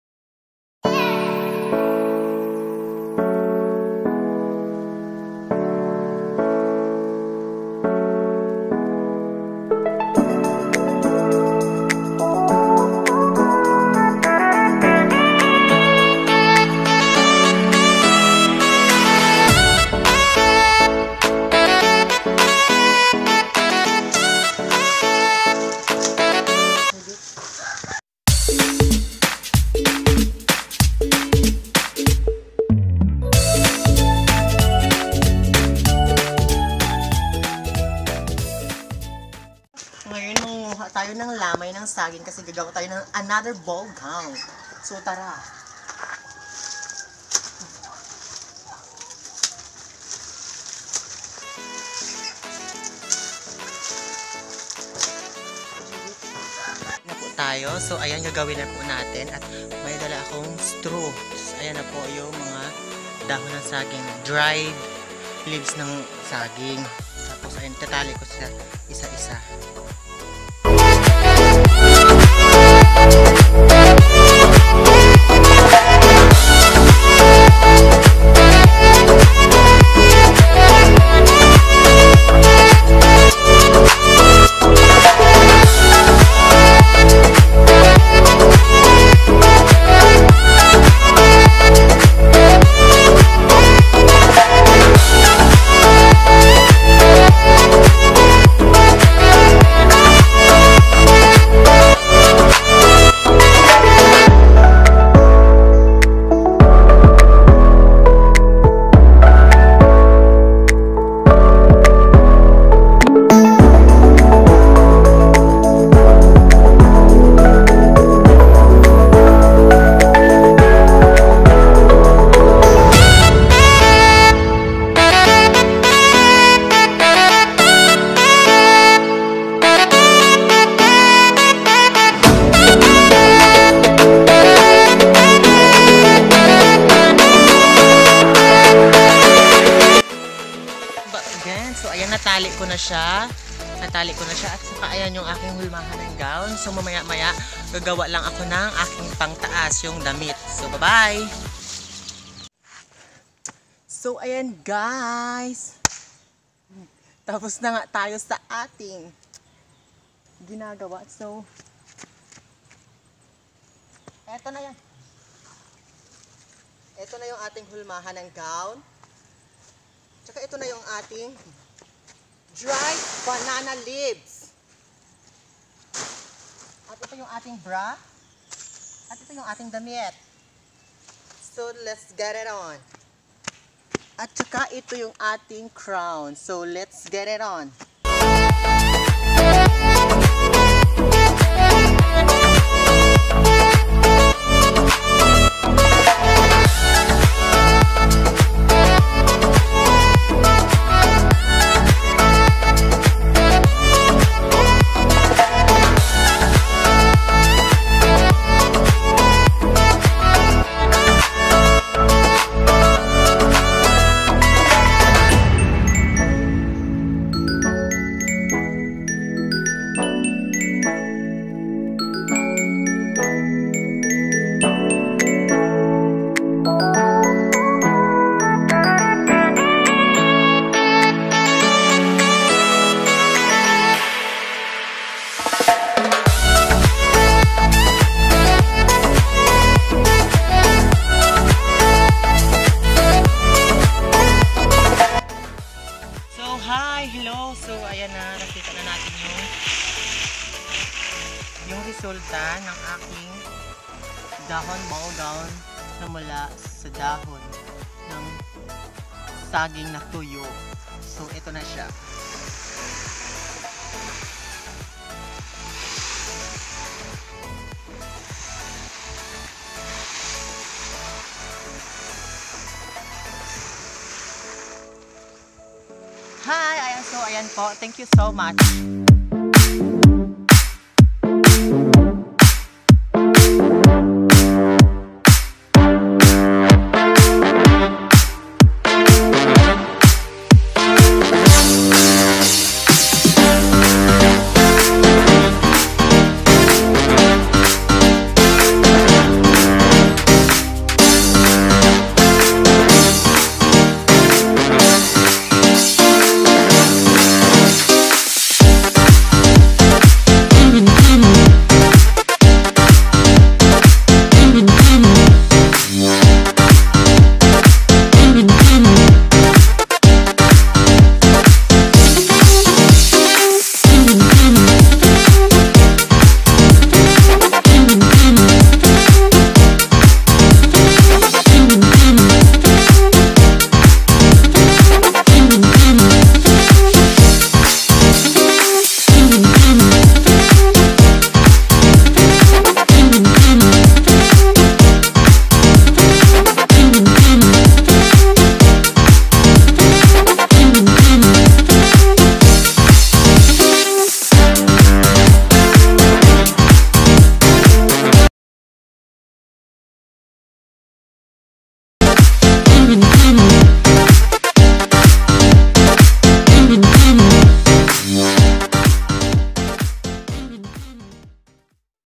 DIY BALL GOWN NA GAWA SA DAHON NG SAGING
Dahon ball gown ,quarantine eddition
Dahon ball gown,boring kaba gawa ka rin ng pagkakalibangan mo